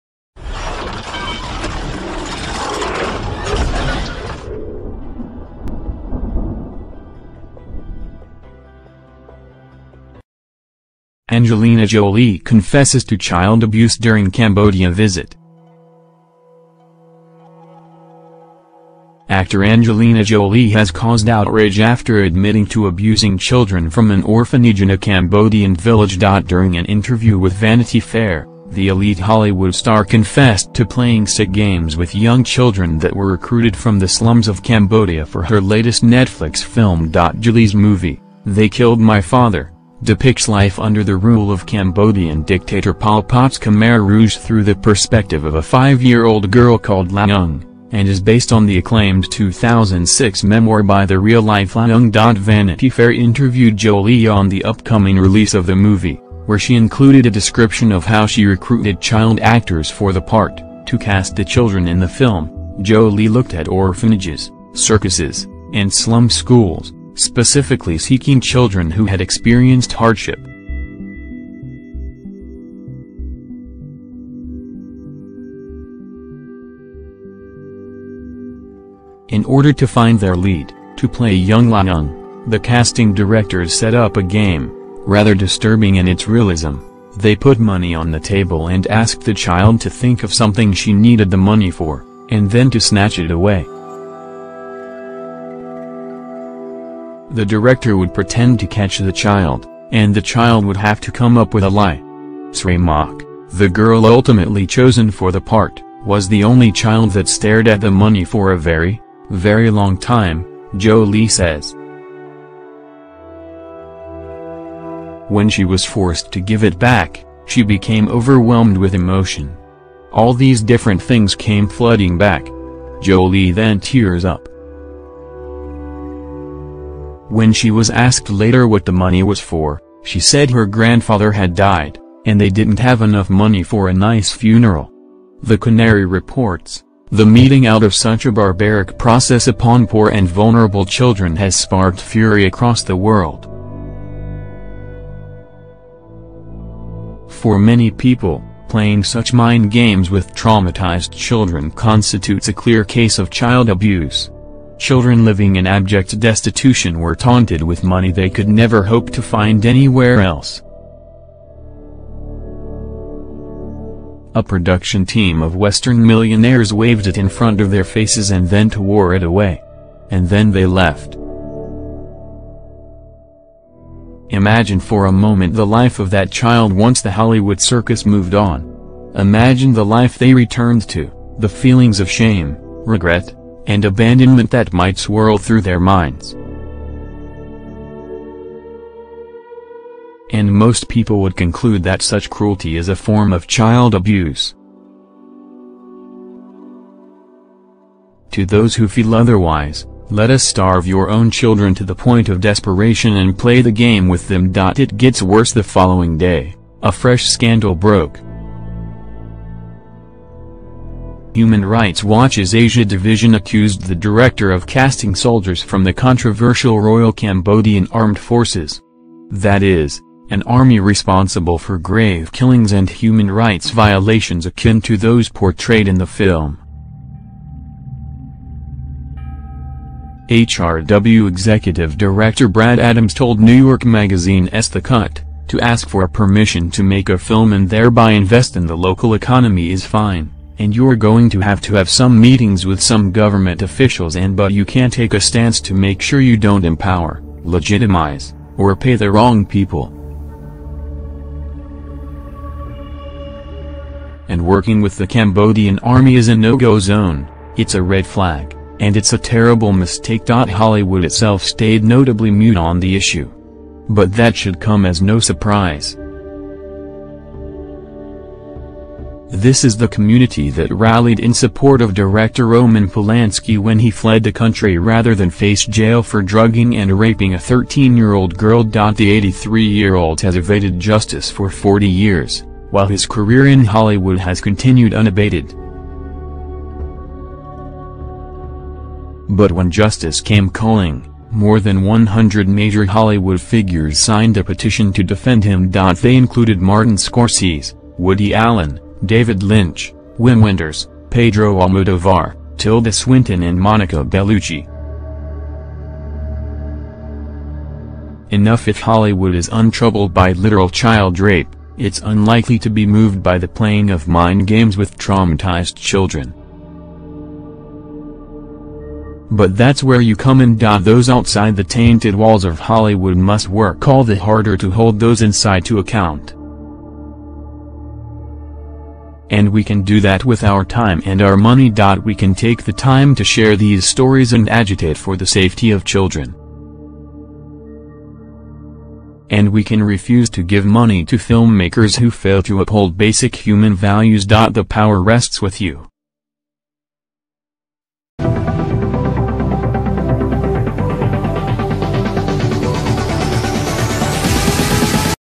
Angelina Jolie Confesses to 'Child Abuse' During Cambodia Visit.
Actor Angelina Jolie has caused outrage after admitting to 'abusing children' from an orphanage in a Cambodian village.During an interview with Vanity Fair, the Elite Hollywood star confessed to playing sick games with young children that were recruited from the slums of Cambodia for her latest Netflix film.Jolie's movie, They Killed My Father, depicts life under the rule of Cambodian dictator Pol Pot’s Khmer Rouge through the perspective of a five-year-old girl called Loung Ung, and is based on the acclaimed 2006 memoir by the real-life Loung Ung.Vanity Fair interviewed Jolie on the upcoming release of the movie, where she included a description of how she recruited child actors for the part: To cast the children in the film, Jolie looked at orphanages, circuses, and slum schools, specifically seeking children who had experienced hardship. In order to find their lead, to play young Loung Ung, the casting directors set up a game, rather disturbing in its realism: they put money on the